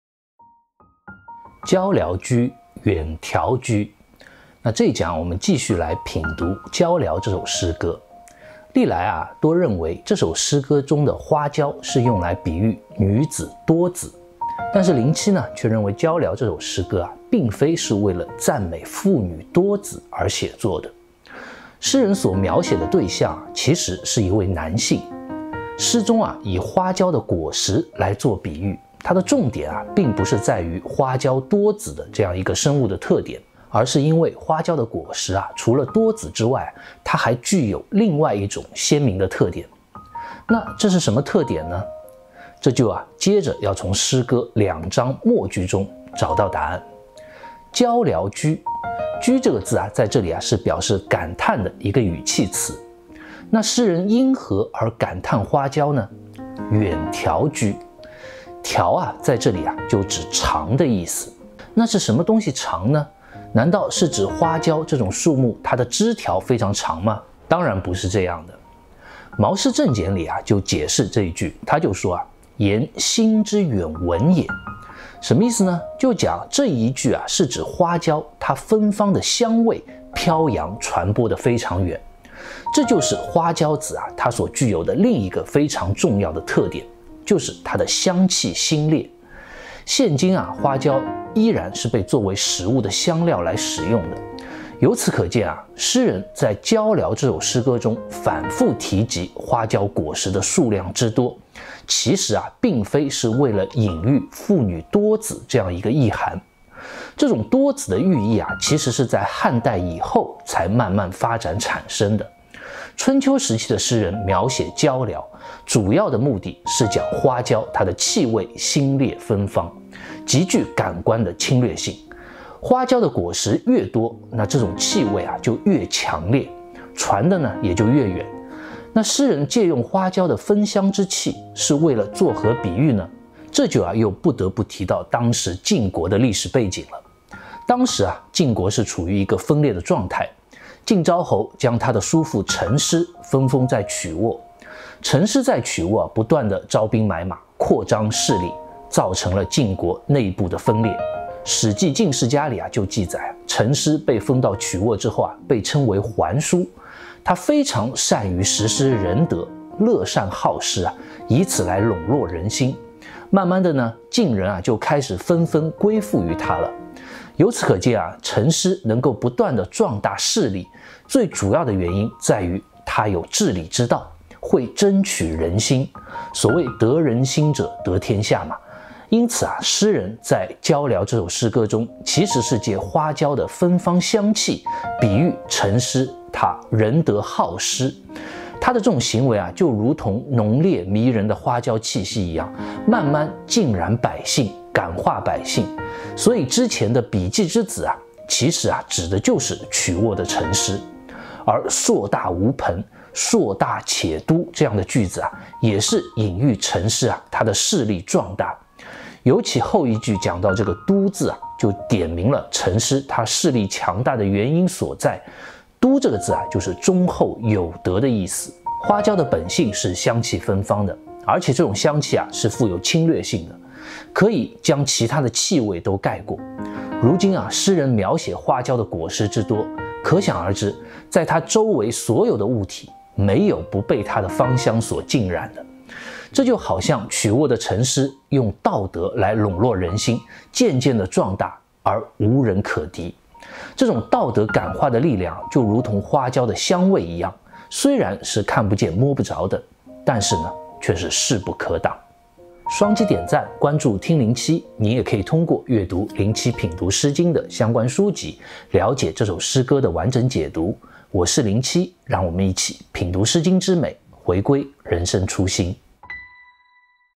《诗经》169 唐风·椒聊 |“椒聊且，远条且”
👍喜欢我的视频别忘记按赞、分享！
                                           唐风·椒聊
椒聊之实，蕃衍盈升。彼其之子，硕大无朋。椒聊且，远条且。
椒聊之实，蕃衍盈匊。彼其之子，硕大且笃。椒聊且，远条且。
                                           唐風·椒聊
椒聊之實，蕃衍盈升。彼其之子，碩大無朋。椒聊且，遠條且。
椒聊之實，蕃衍盈匊。彼其之子，碩大且篤。椒聊且，遠條且。

【听林栖】我们一起读诗经
品读诗经之美，回归人生初心
【更多精彩视频】